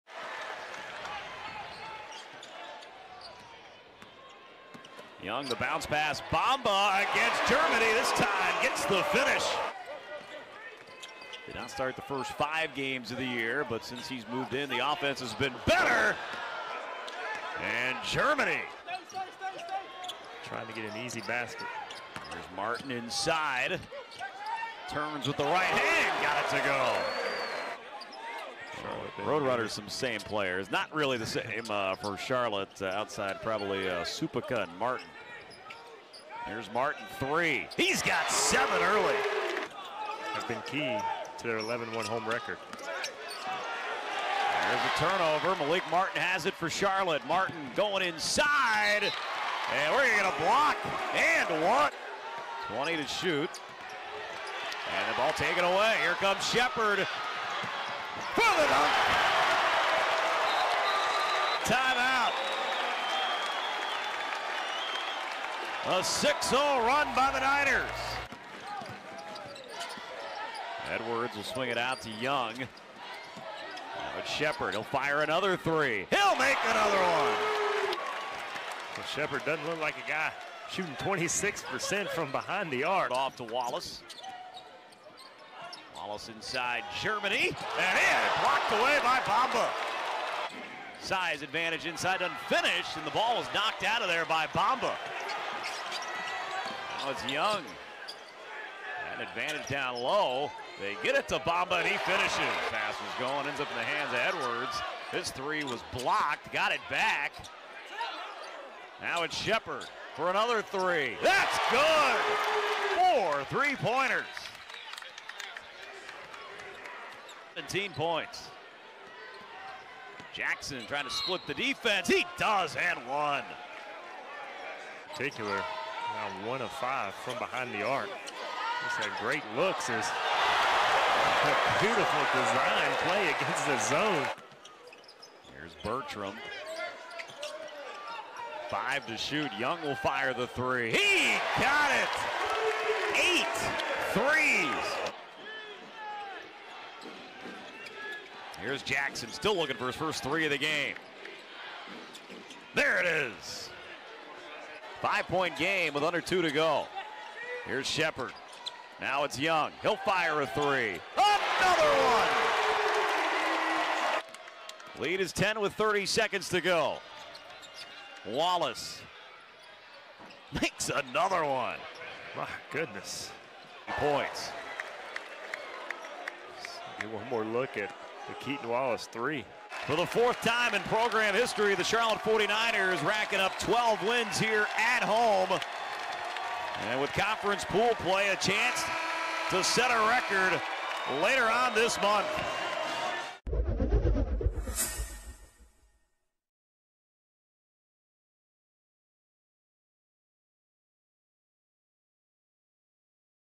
UTSA vs. Charlotte Basketball Highlights (2019-20) | Stadium
Charlotte stayed hot in the Queen City as the 49ers won their ninth-straight game at home with a 91-84 win over UTSA on Saturday afternoon. You can find all the highlights from this conference clash right here!